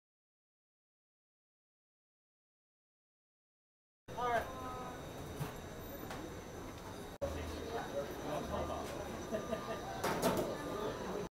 Joe'sSteamRiceRoll booth in Flushing
Joe's Steam Rice Roll booth in a tiny mall in Flushing is one of my faves. Freshly made and ready to go in a few minutes. On weekend, it may take as long as 20 minutes.